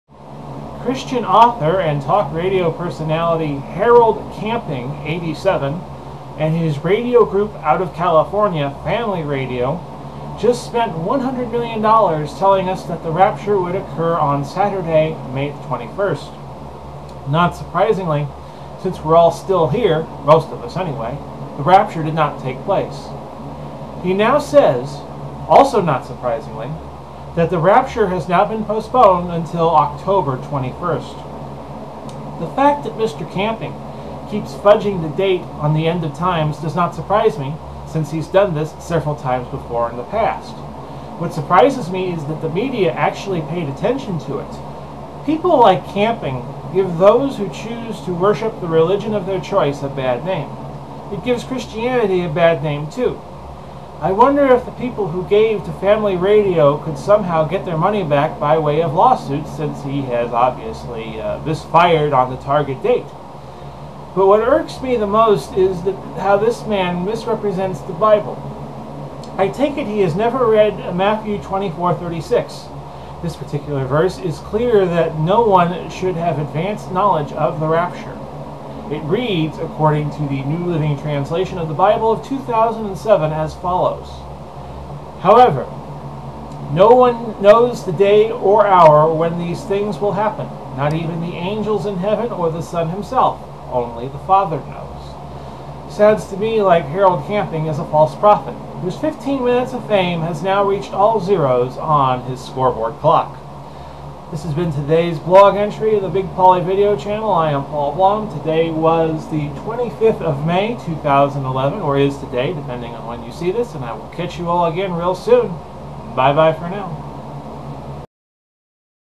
Postponing The Rapture
Thoughts on the May 21, 2011 rapture that wasn't.